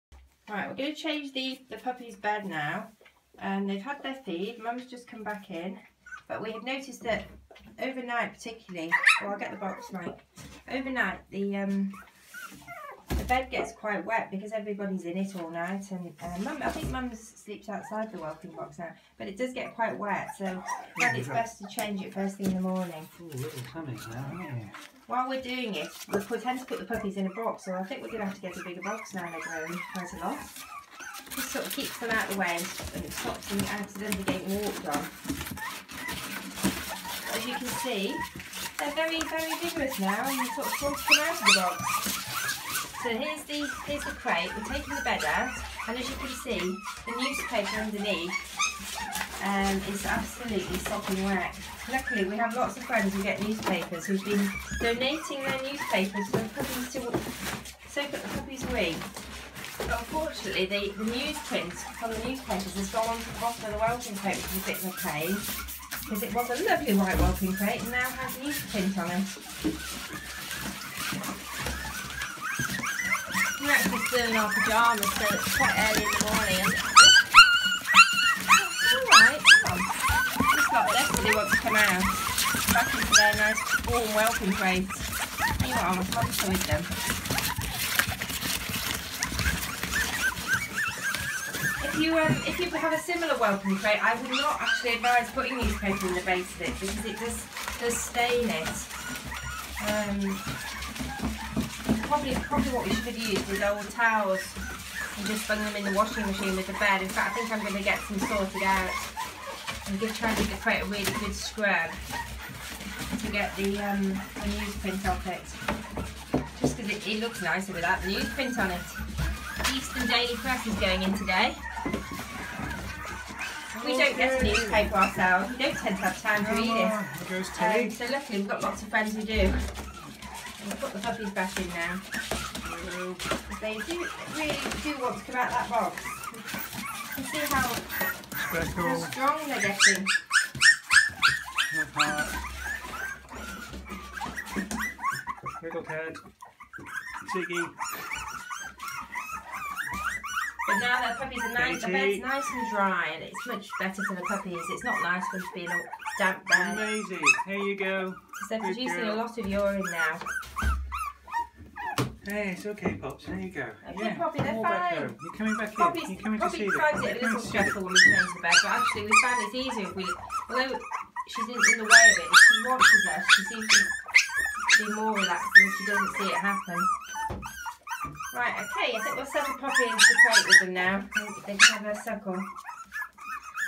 How often to change the puppy's bedding
The puppies are nearly 4 weeks old. In this video we show you how we change the bedding and offer tips on different types of bedding and how often to change it .